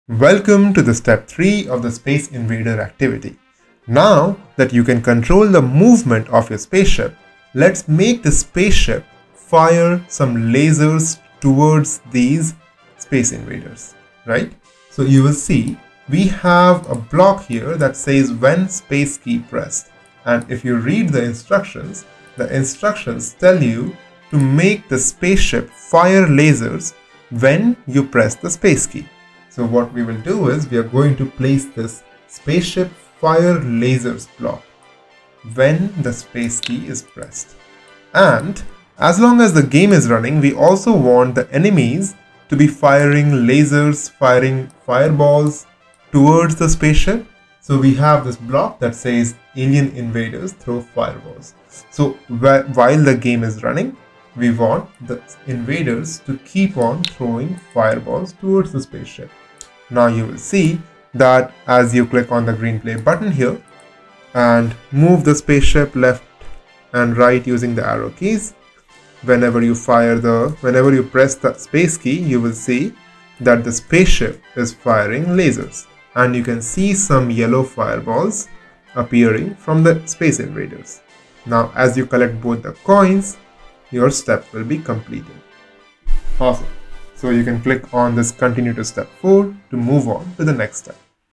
Space Invaders - Hour Of Code - Step 3 Walkthrough
Code the classic space invaders game in 3D, while learning about the basics of game development, and motion in 3D space and learn fundamental programming concepts like loops, conditionals, variables, and keyboard events, all in less than an hour.